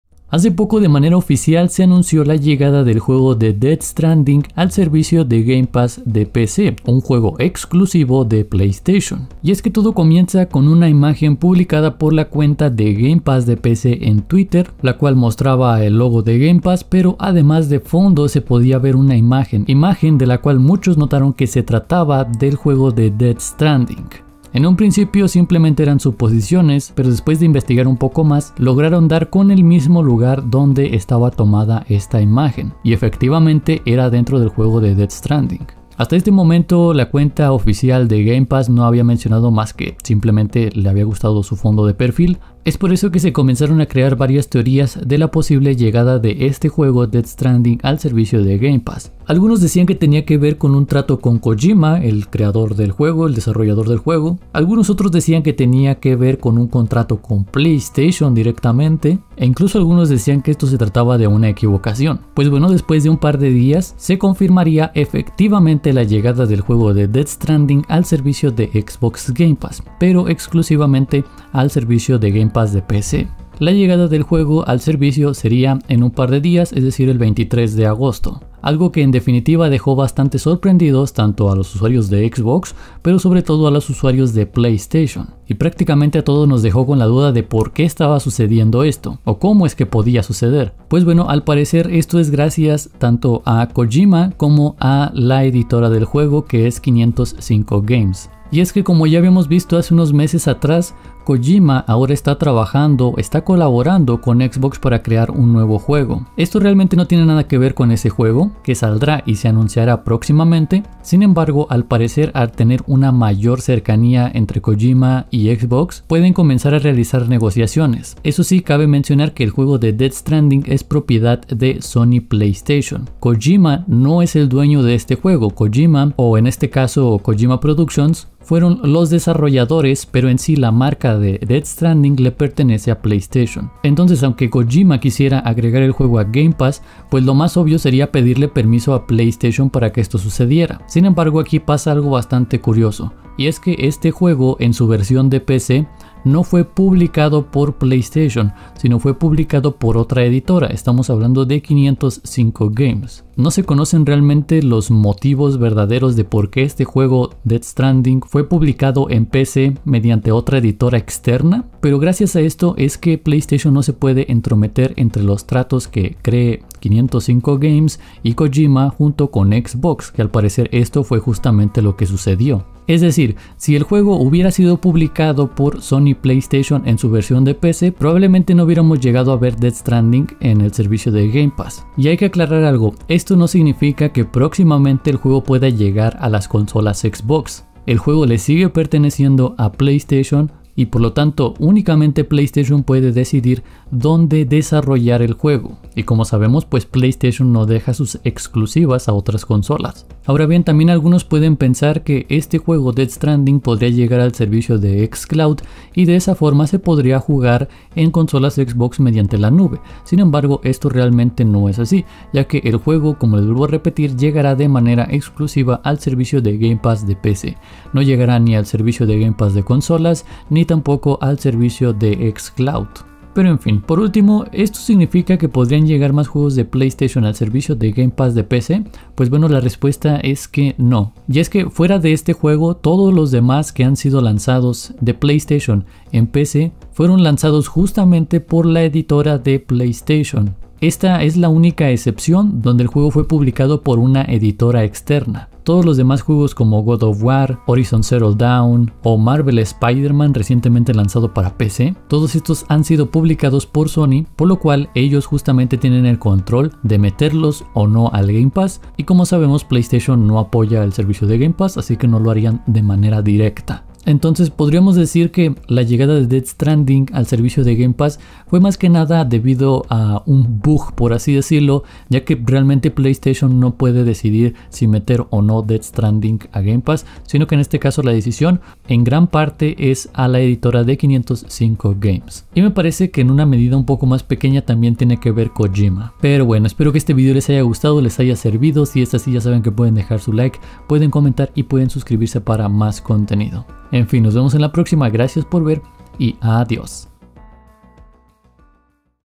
🎮 Death Stranding en Game Pass es Oficial pero ¿POR QUE?
En este vídeo les hablo sobre el tema de death stranding en el servicio de xbox game pass...